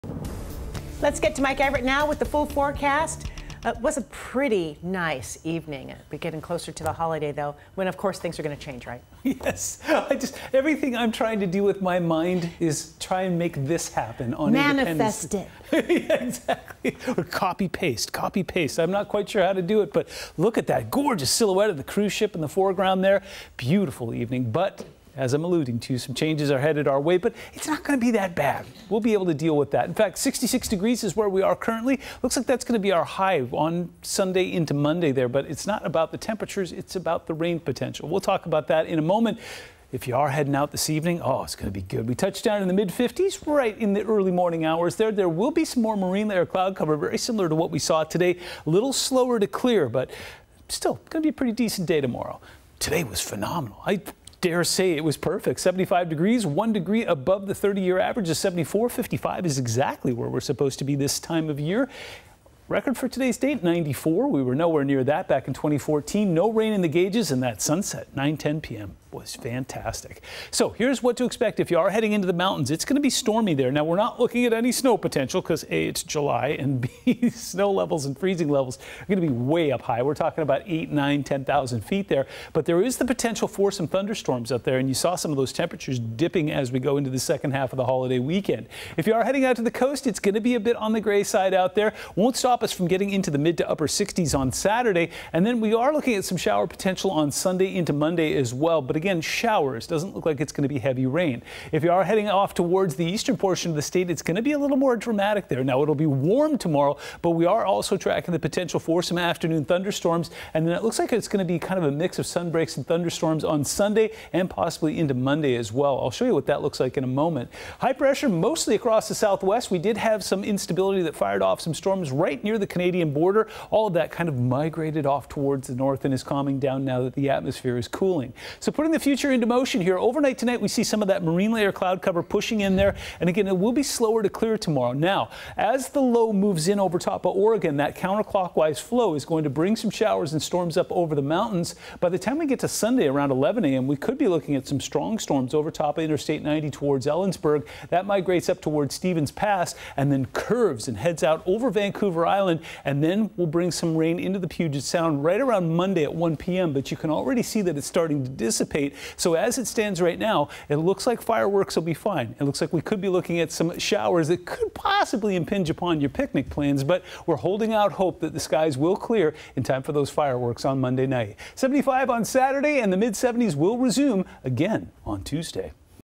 KING 5 Weather
KING 5 Chief Meteorologist Mike Everett gives an update on the late evening forecast for 07/01/22.